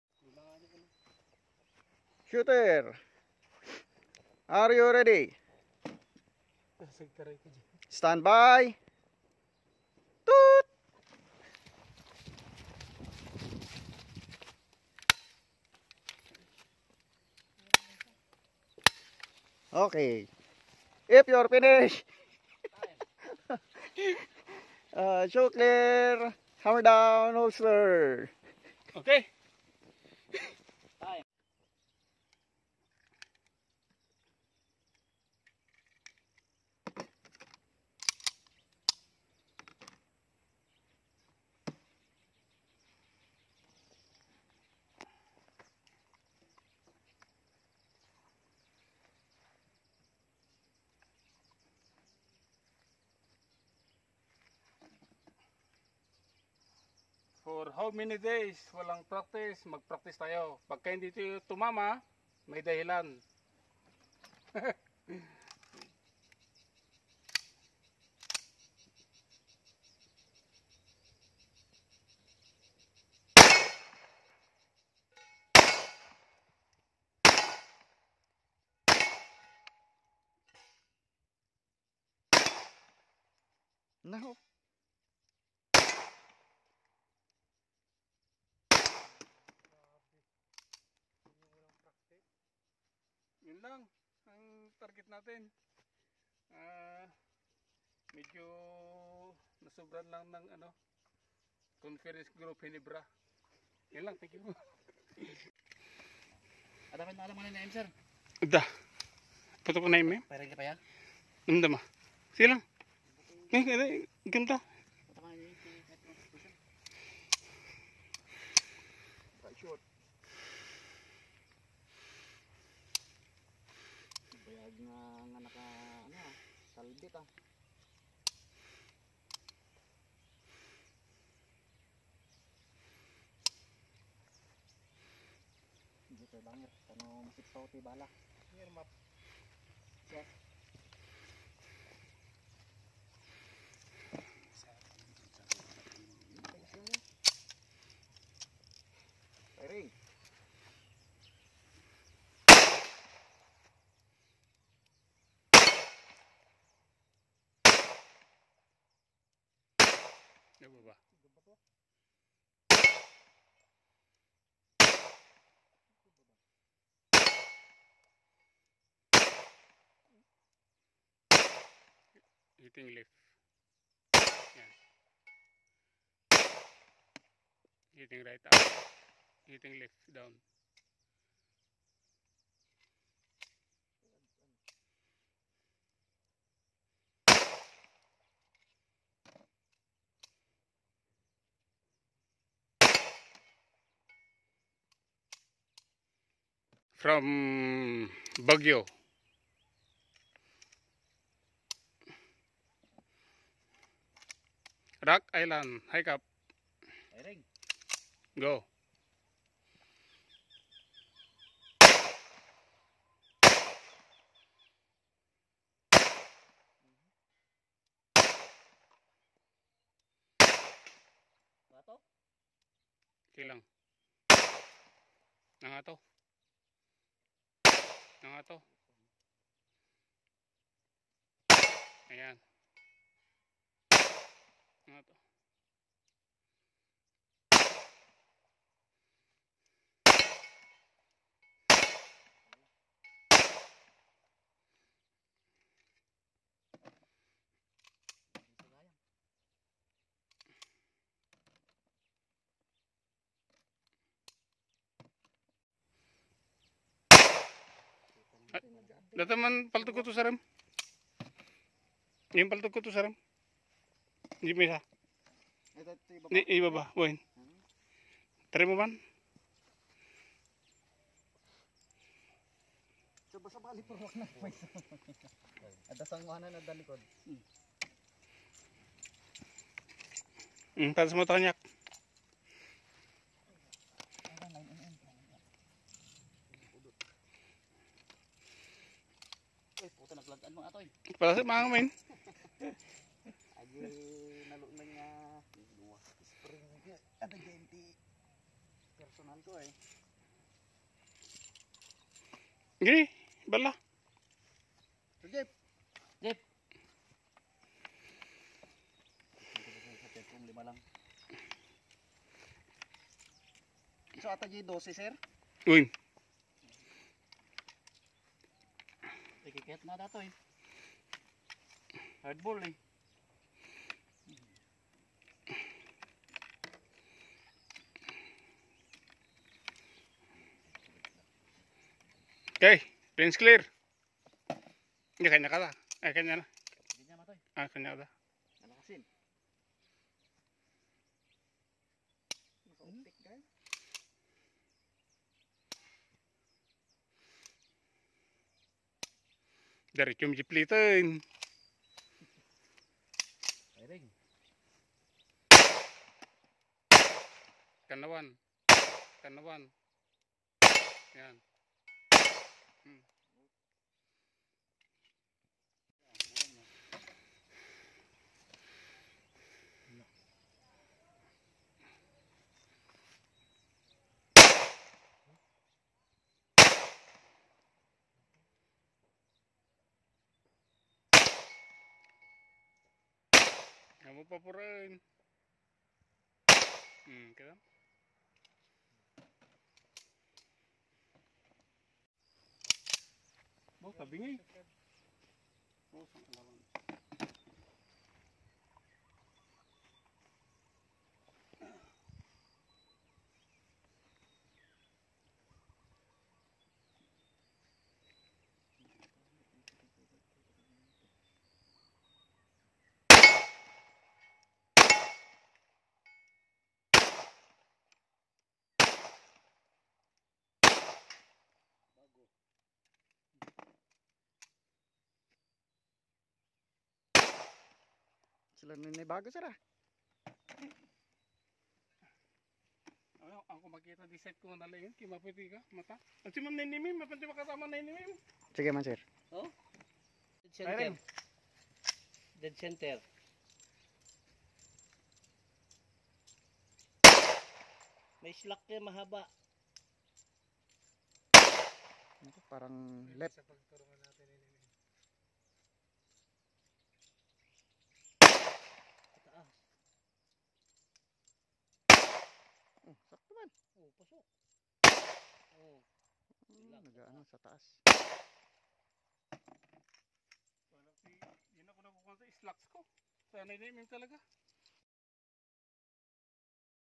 Steel Challenge and Fun Shoots after the Pistol Marksmanship
After our pistol marksmanship activity, we decided with our friends to take the hitting the plate challenge with an estimated distance of 25 meters.